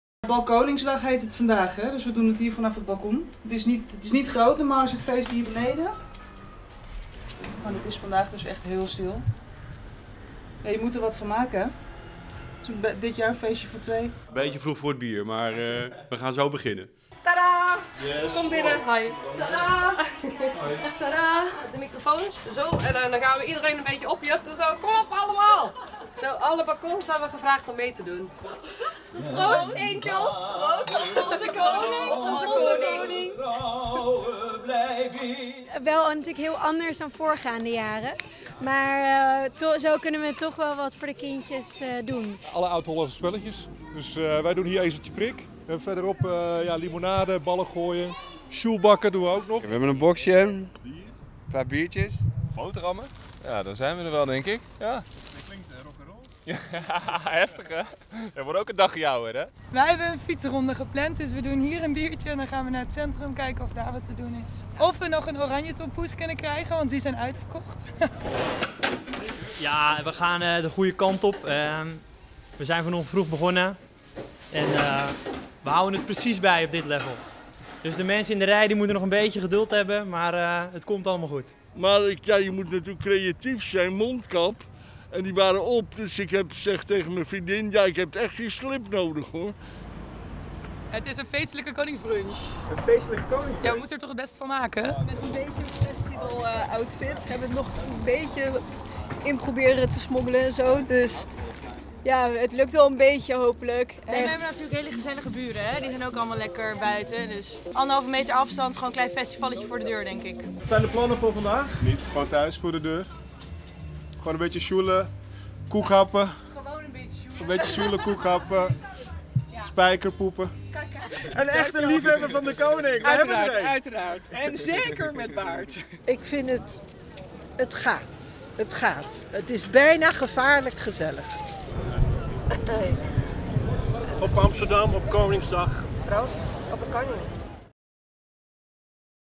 Zo vieren Amsterdammers Koningsdag 2020
Amsterdam viert vandaag een bijzonder aparte editie van Koningsdag. Wij namen een kijkje in de stad om te zien hoe Amsterdammers deze 'Woningsdag' beleven.